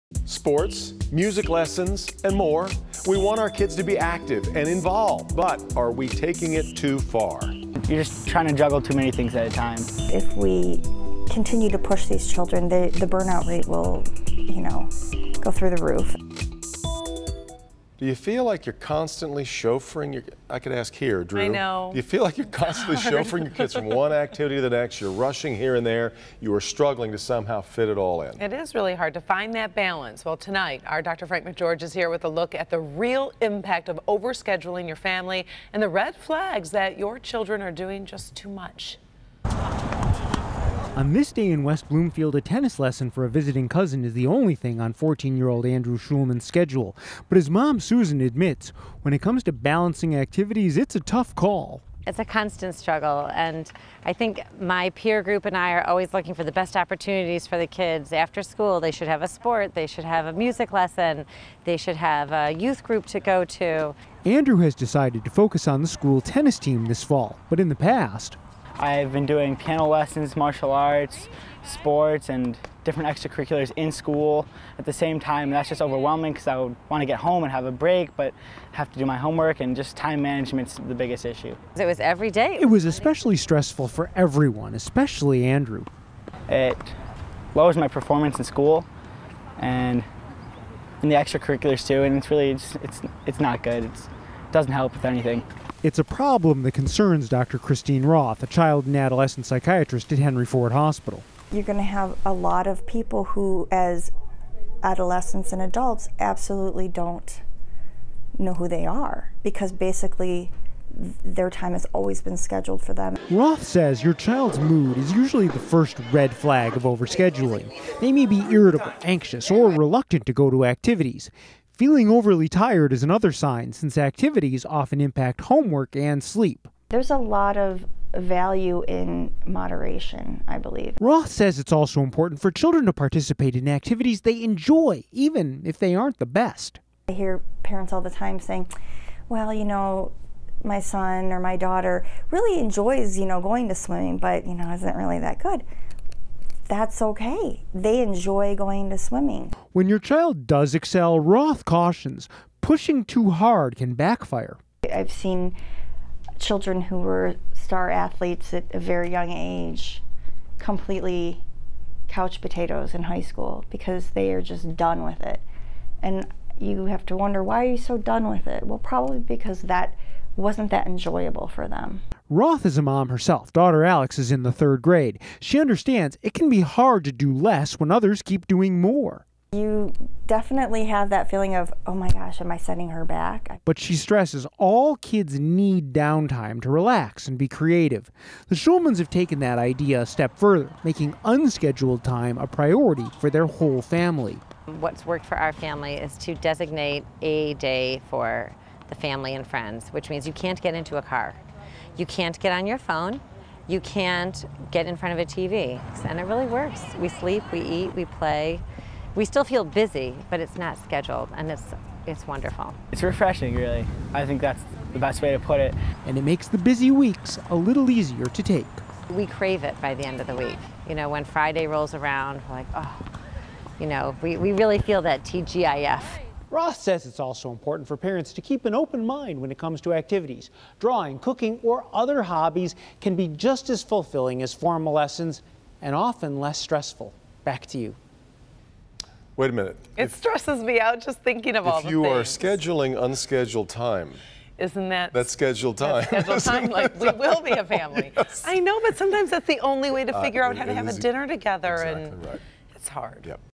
Good Health: Are your kids overscheduled?
Sports, music lessons and more. We want our kids to be active and involved, but are we taking it too far?